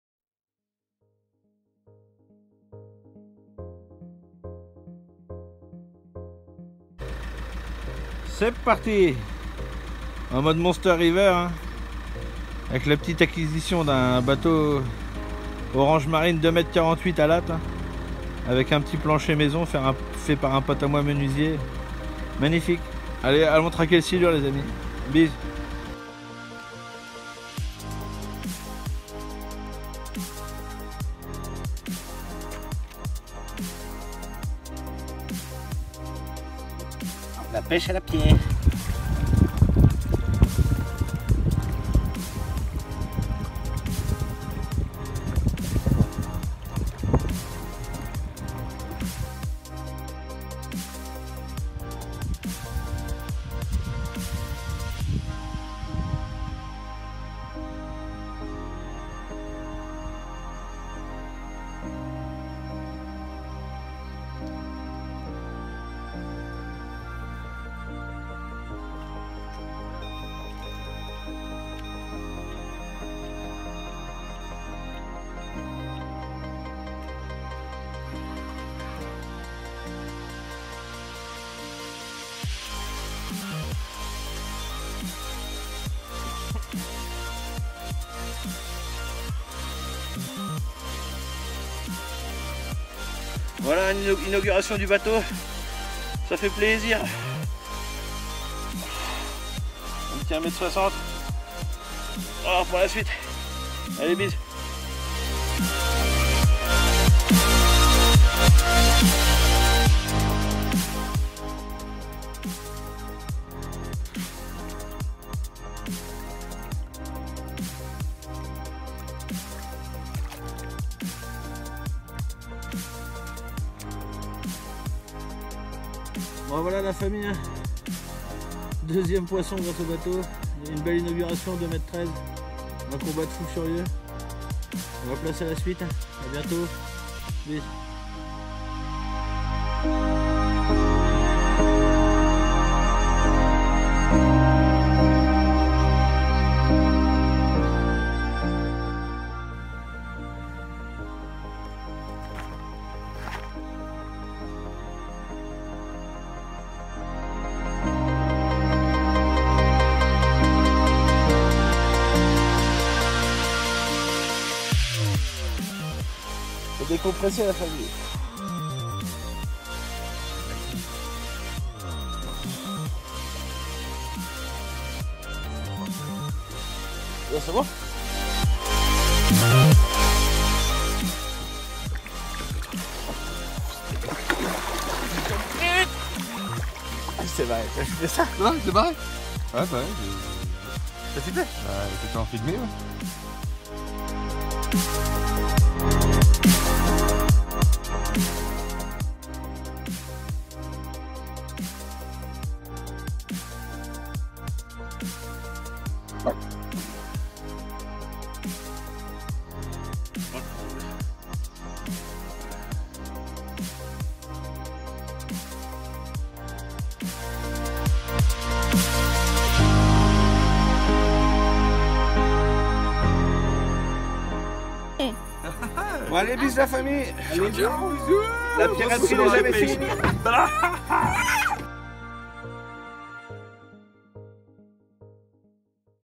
La pêche du Silure à la pierre ! Vol : 2
Salut a tous , voici ma nouvelle vidéo inaugurant l achat d un petit bateau !
Voici mes premiers pas en t an que capitaine , la traque du silure a la pierre et aux cassants vol:2 c partie !
Des superbes images qui change pour ma part , bonne visu a tous !
Je suis de retour , biz